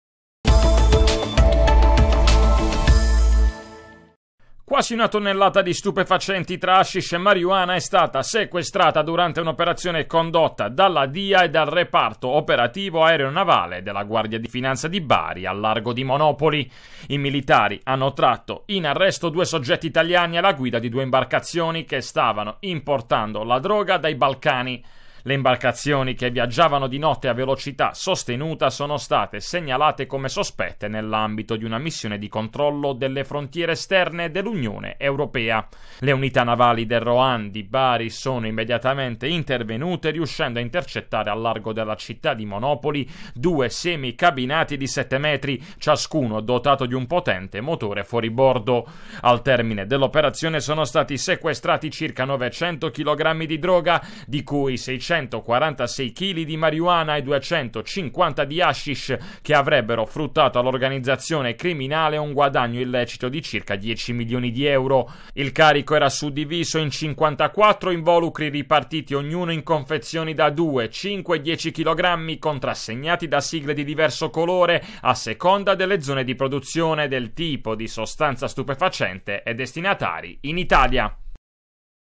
MONOPOLI | Sequestrata una tonnellata di droga a largo di Monopoli: 2 arresti
Intercettata un'imbarcazione dalla Dia e dalla Gdf: a bordo hashish e marijuana